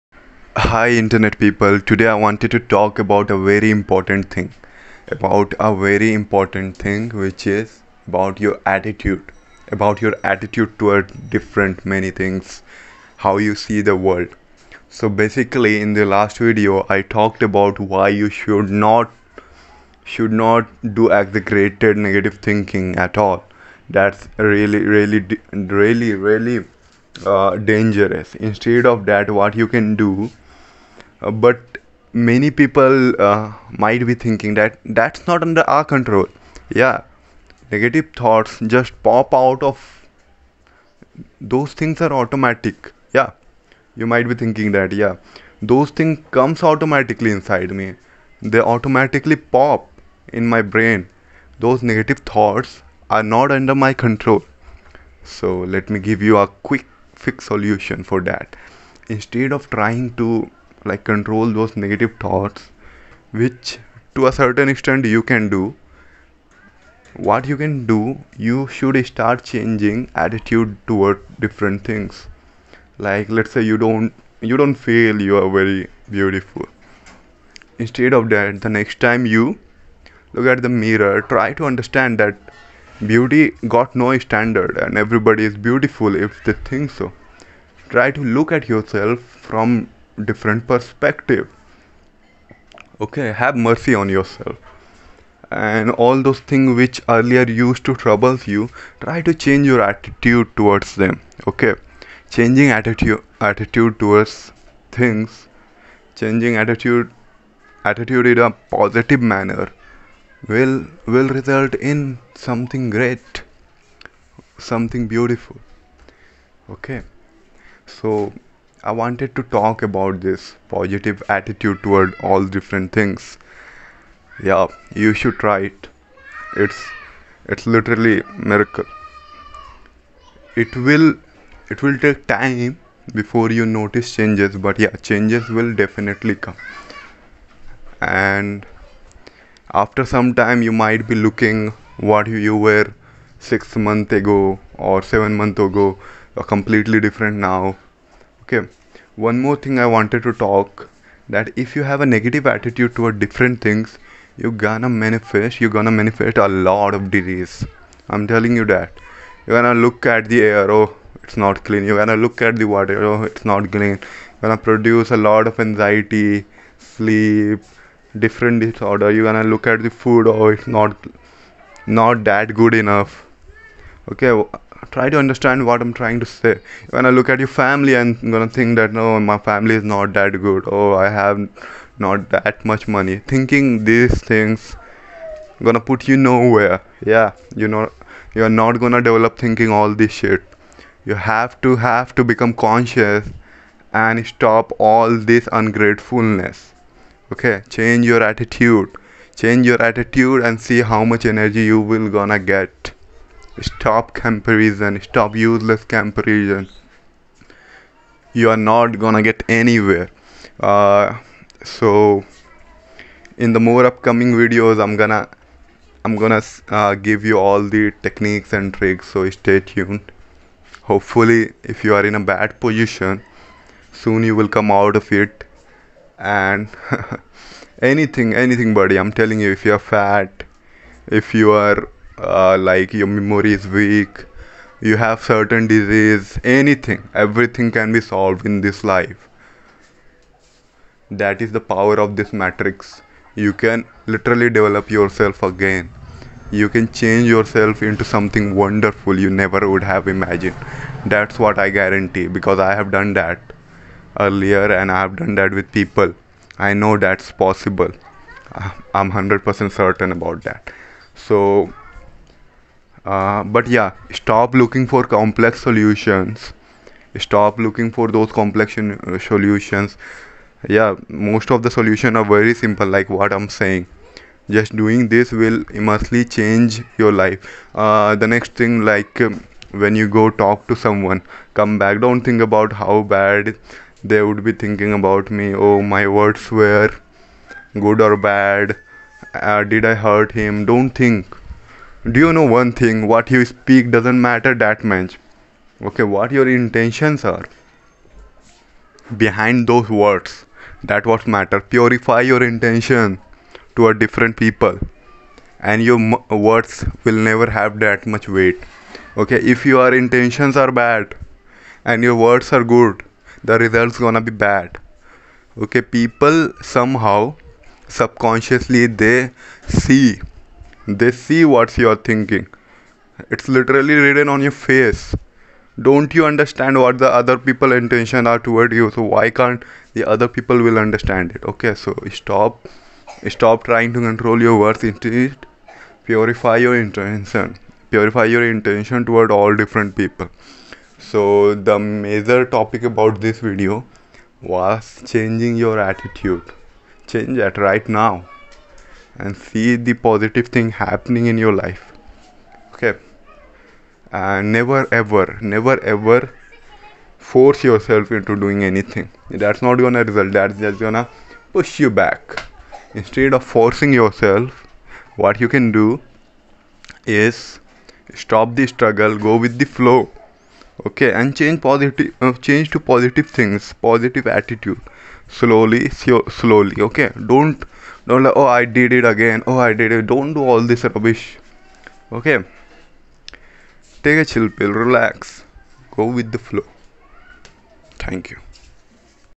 The Power of Changing To Positive Attitude (Works 100% Guaranteed)
About this subliminal:

✨ I NEVER include negative affirmations in my subs so you guys can listen to this without any worries!

✨ This subliminal is short and the affirmations are sped up (not by a lot) so that you can listen to it a lot and it still makes sense.

IMPORTANT:

✨ Please comment any results you get! This might not seem like a lot to ask but it will help anyone who is thinking about using this sub and it helps me too!

I do not like doing self-promos but since this is a new account, it is hard to gain publicity and I spend a lot of time making these subs. Thanks!